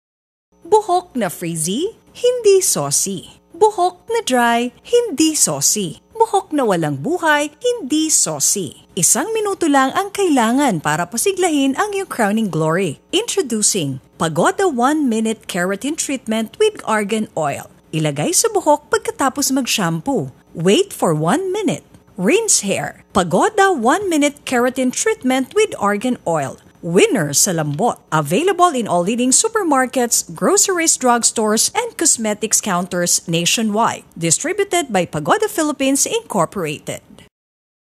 Pagoda 1 Minute Keratin Treatment Hot Oil Radio Commercial (2016)
Pagoda 1 Minute Keratin Treatment with Argan Oil: Winner sa lambot!
Available in all leading supermarkets, groceries, drugstores and cosmetics counters nationwide
Distributed by Pagoda Philippines Inc.